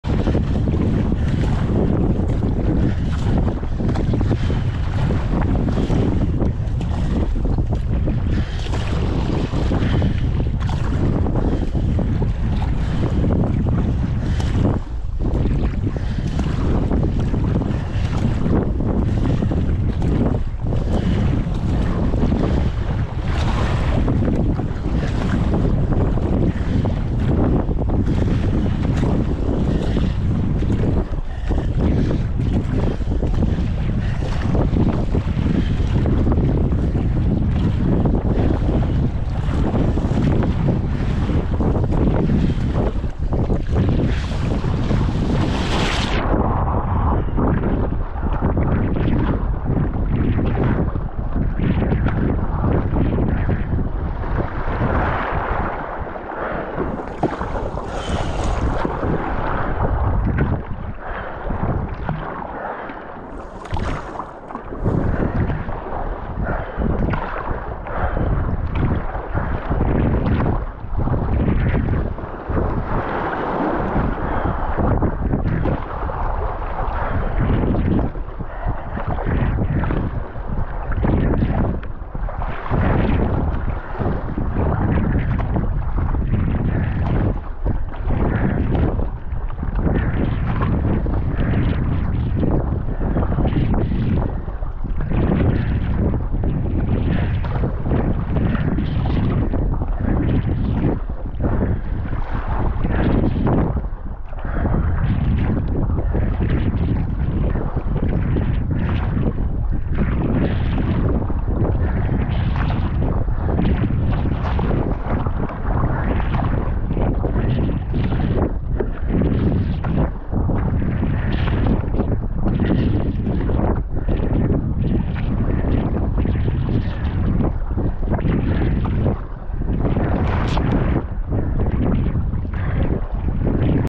Safety boat - 2023 Vaikobi King and Queen of the Harbour
Thanks to all the crew in the safety boat making the race safe! Tough race.